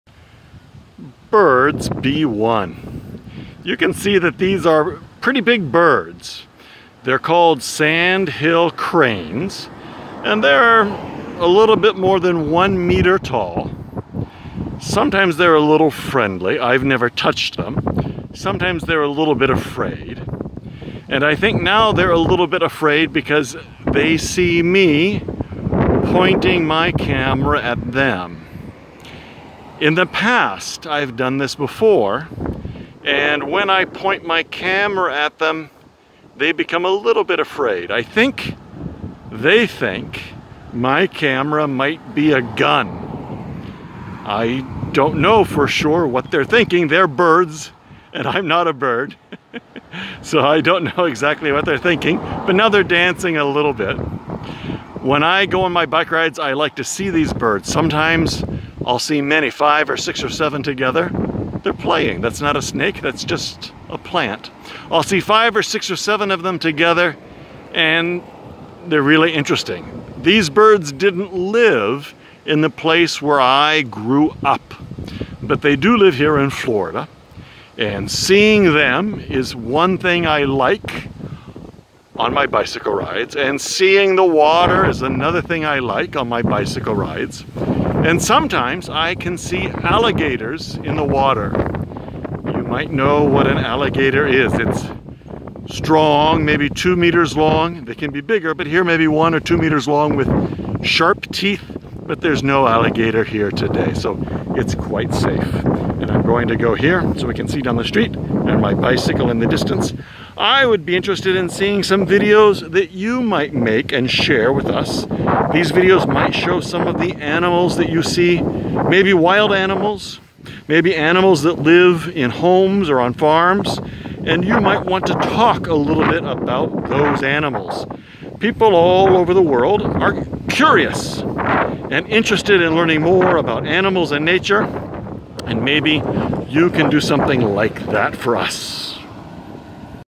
Birds - B1
This is a short video for TESOL and ESL. It's at a level of B1 (intermediate)